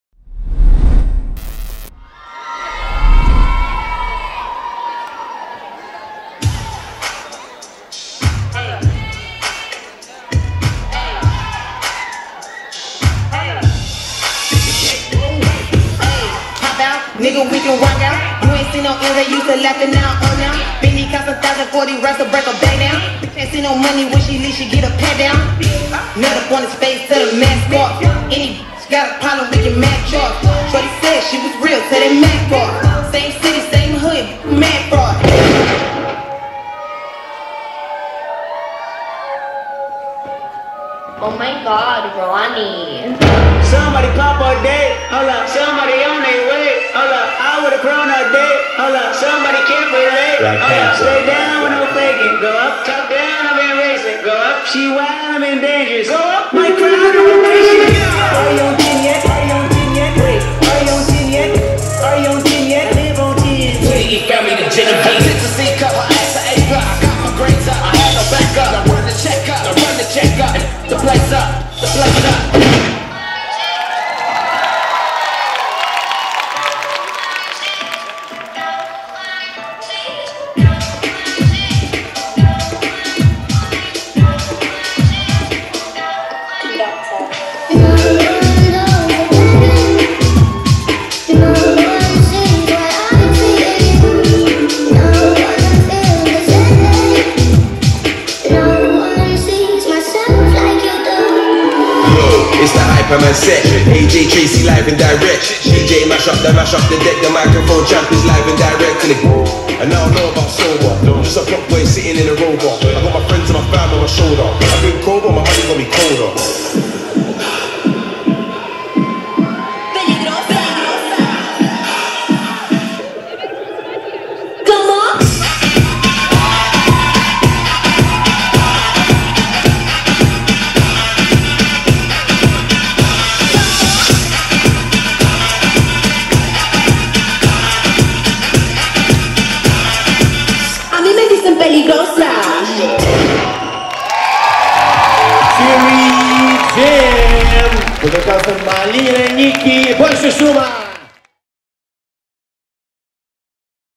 Cherry Jam на зимнем отчетном концерте 2019
Выступление команды Cherry Jam, представляющей студию ART FORCE Кубинка.